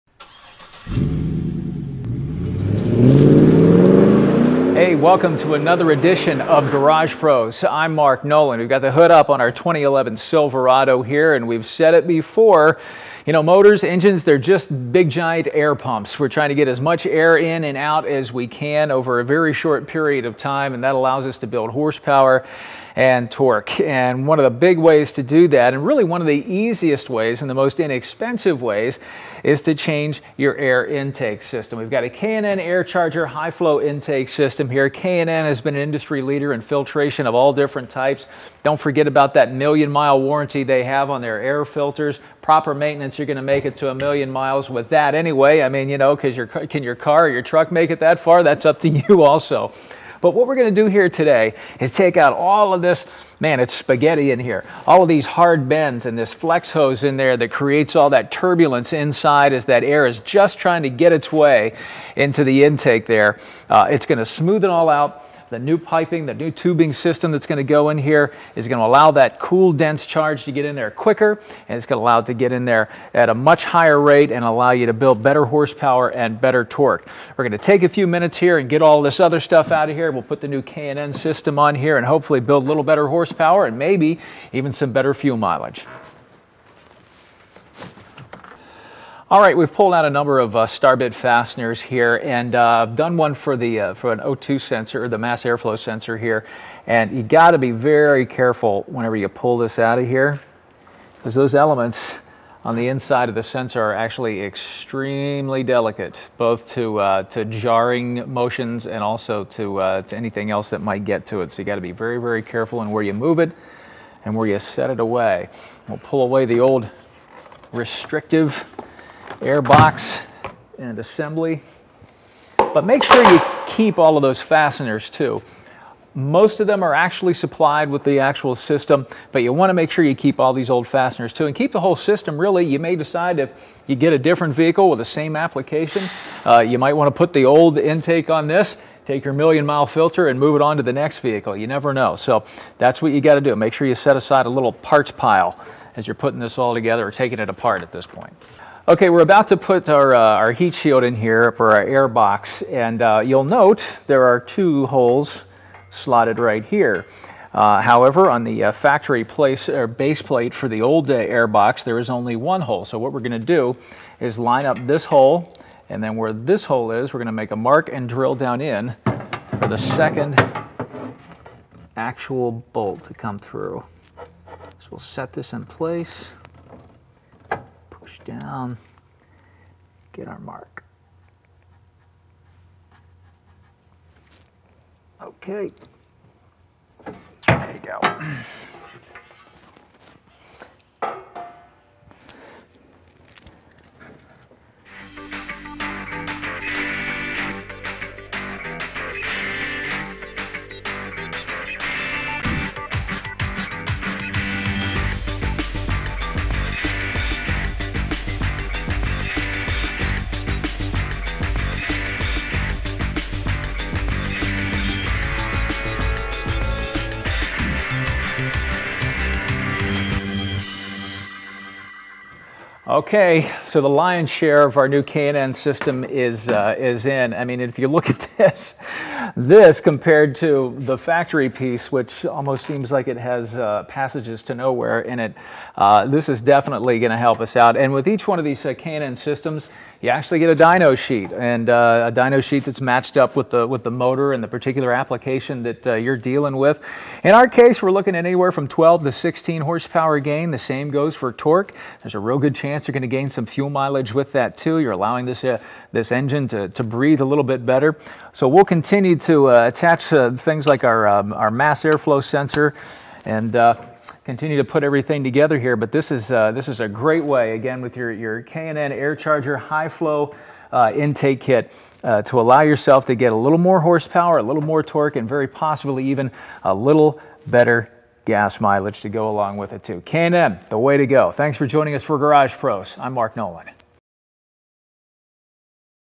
2011 Chevy Silverado K&N High Flow Intake Kit Installation K33573070
Mark Nolan of GragePros shows you how to install the K&N High Flow Intake Kit and explains the advantages of using one over factory air intake systems.